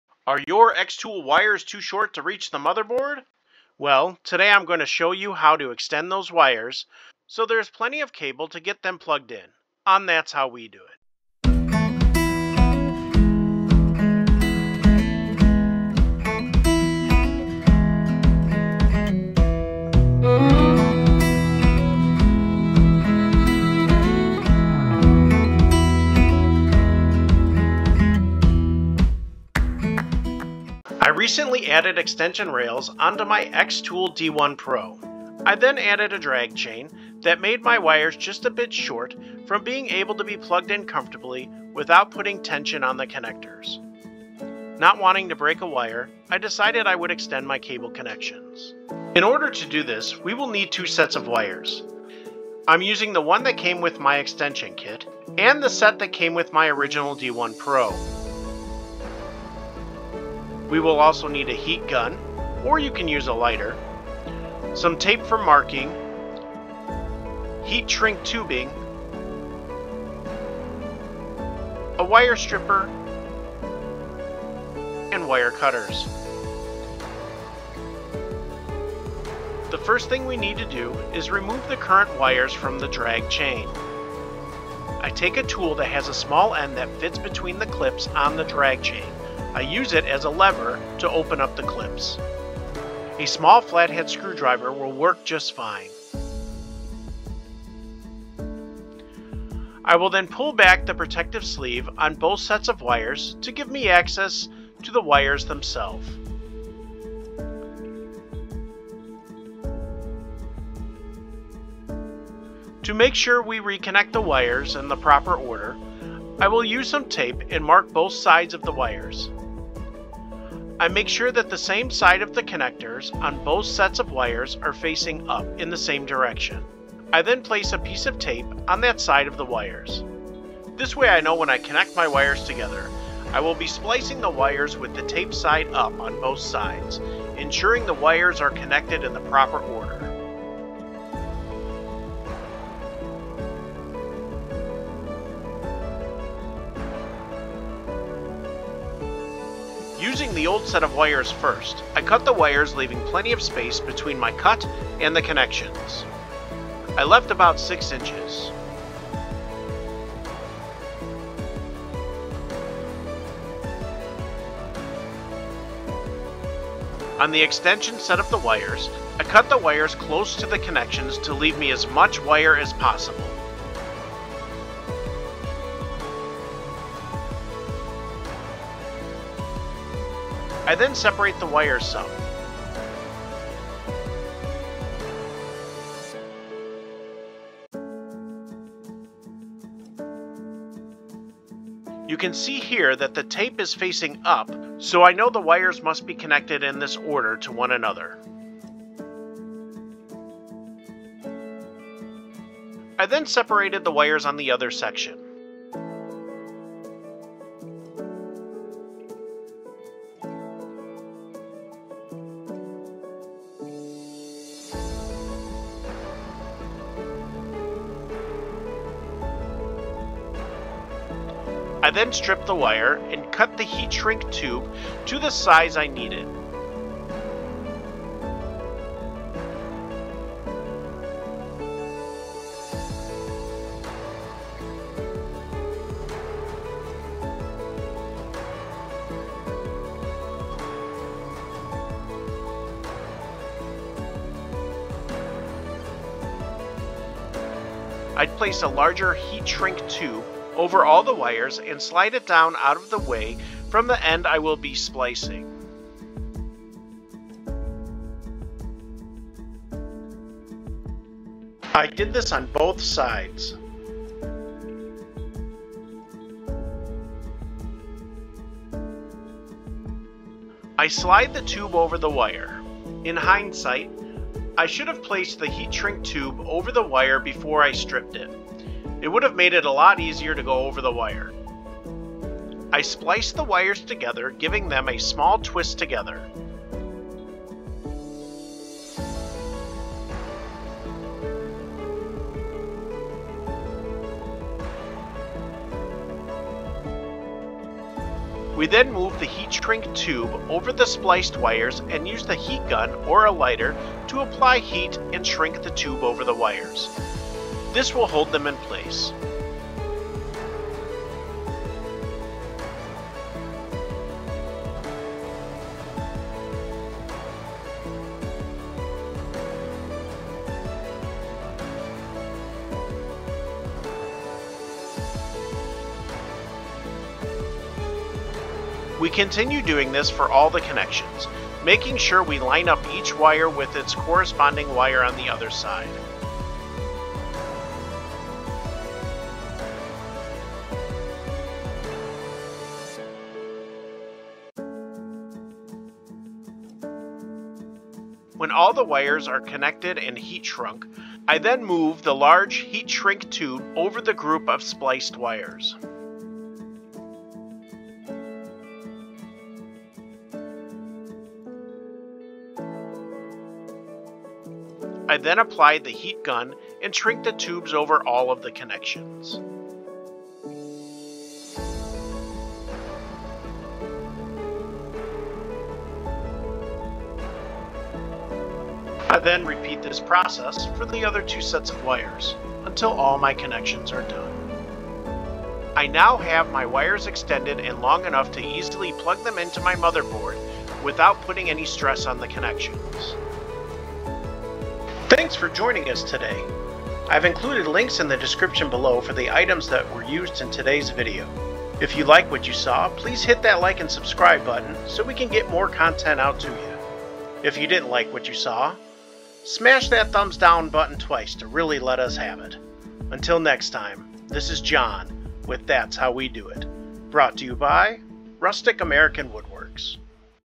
Xtool Wire Extension - How to Extend your Xtool wires so they fit nicely into your motherboard
I recently extended my Xtool D1 Pro and added a drag chain.  I found that my wires were now stretched, leaving a lot of stress on my connections.  So, I decided to extend my wires to alleviate the stress before my connections broke.  All items in this video have been self-purchased.

Heat Shrink Tubes

Wagner Heat Gun

Mulit Tool, Wire Stripper, Wire Cutters, Crimper Tool

Honeycomb laser bed Links 19.68”x37.4”x0.87” Just as good as Xtool for a much lower cost